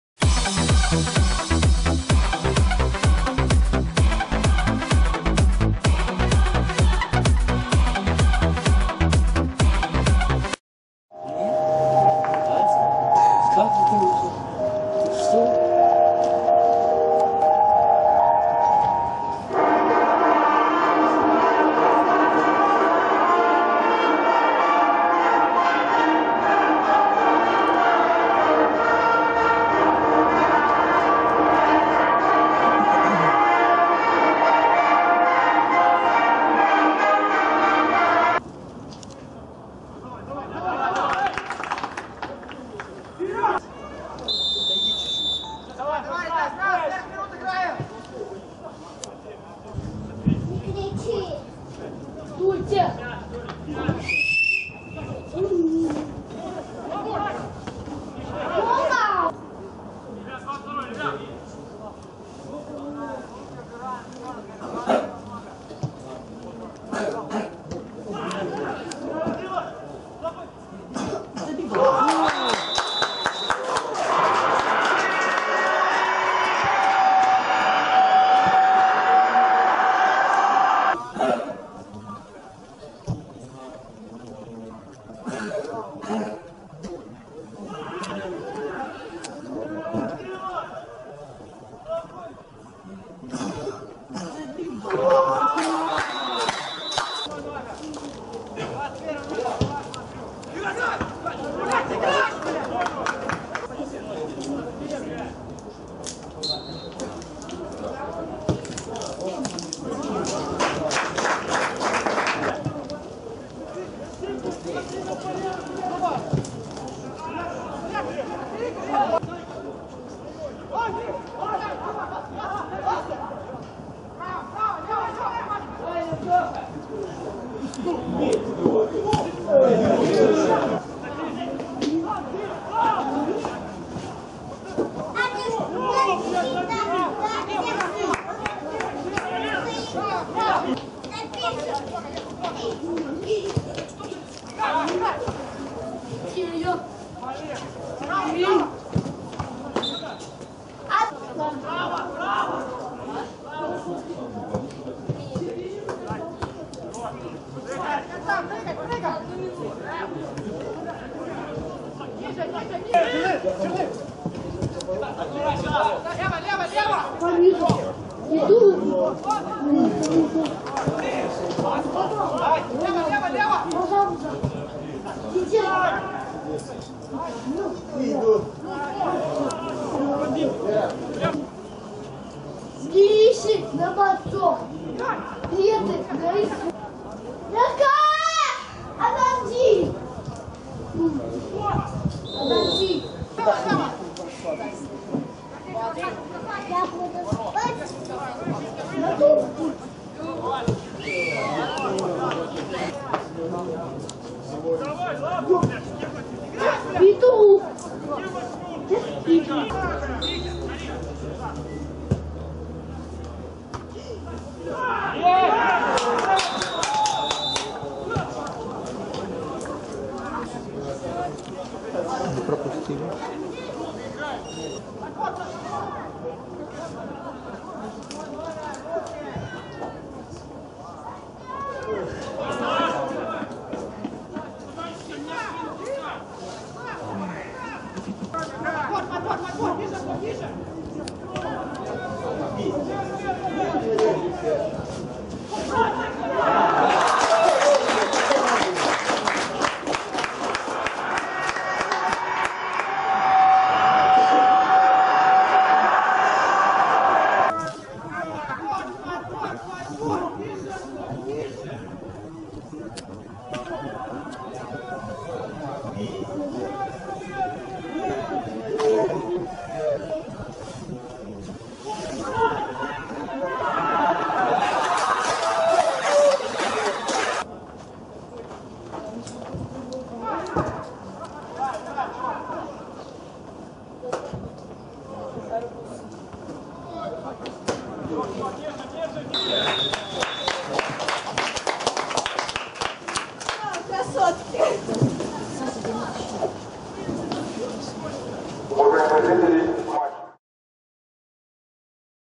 Гомель 2-1 Белшина (дублирующие составы) 29.03.2014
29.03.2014
Обзор матча чемпионата Беларуси по футболу среди команд дублеров. ФК "Гомель" - Белшина 2-1